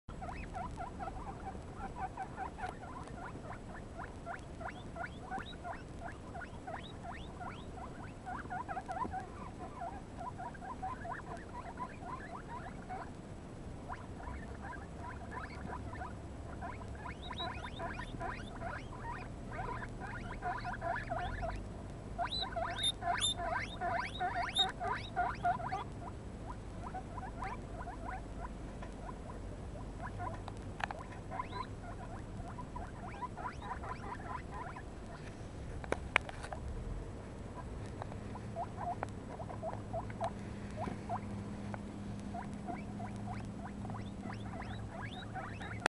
los 3 enanos en los puzzles de numeros
de verlos por los foros me acordé que yo también tenía un pequeño puzzle de números, así que lo aproveché e hice un minicorralillo para los 3 pequeñajos y que corriesen un poquito. Son tan peques que no necesitan mucho espacio, jejeje.